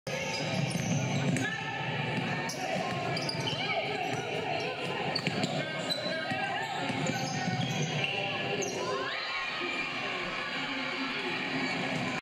PORTIA ZALEEN #1 (V-CAPTAIN) SLICES TO THE BASKET FOR A CLEAN FLOATER 💯🇵🇷🇺🇸✔
PZB #1 (V-CAPTAIN) SLICES TO THE BASKET FOR A CLEAN FLOATER 💯🇵🇷🇺🇸✔ vs CHELMSFORD GAME #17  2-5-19